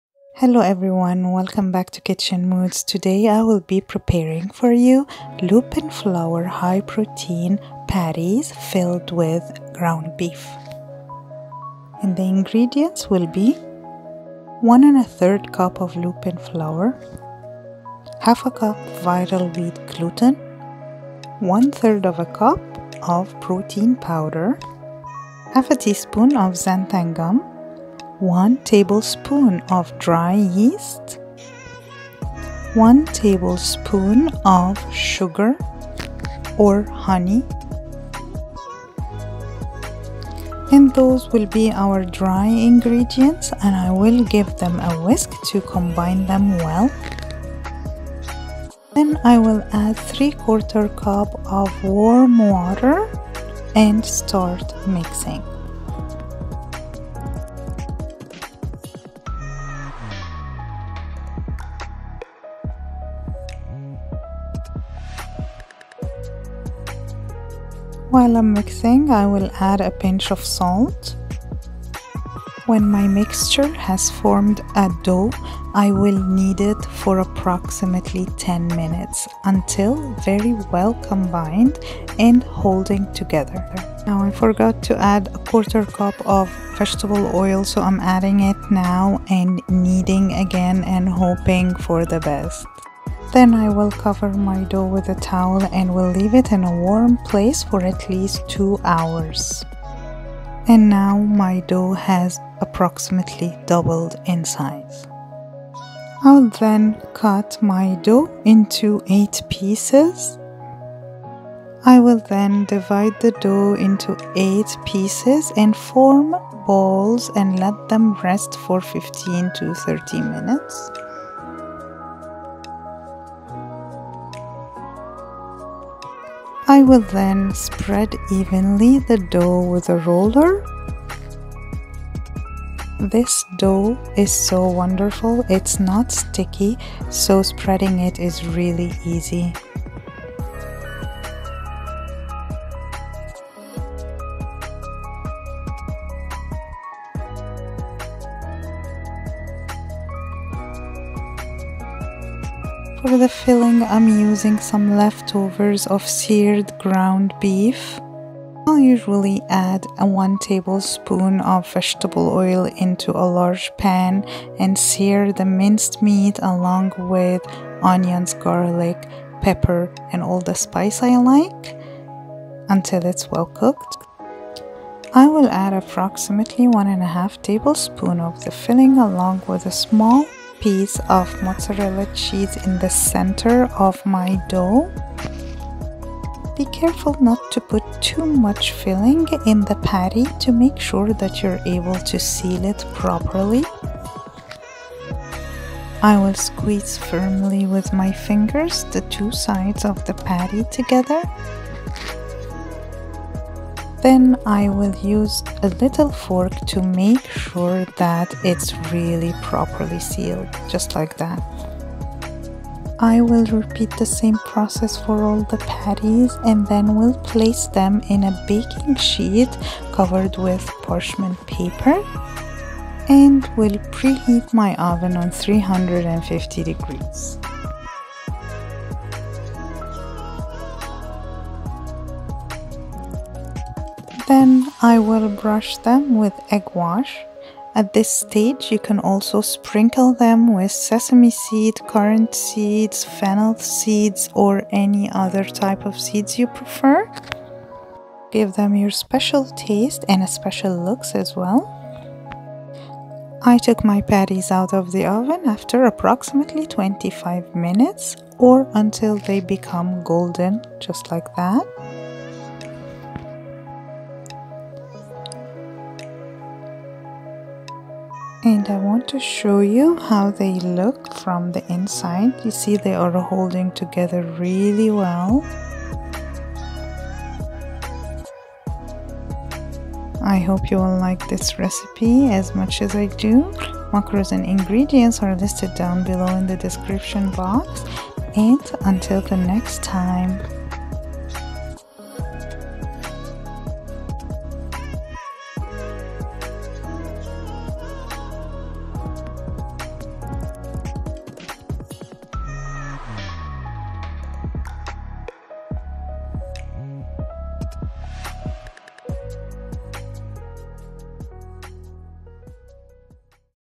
High Protein Lupin Flour Beef Patties
Ingredients:
1 1/3 cup lupin flour
1/2 cup vital wheat gluten
1/3 cup protein powder
1/4 cup oil
3/4 cup warm water
1 tbsp yeast
1 tbsp sugar
Pinch salt

Filling:
150 g ground beef
onion, garlic, meat spice
50 g favourite cheese

Macros per patty (total 8 patties):
Net carbs: 3 g
Protein: 24 g
Fat: 8 g
Calories: 200

Musician: @iksonmusic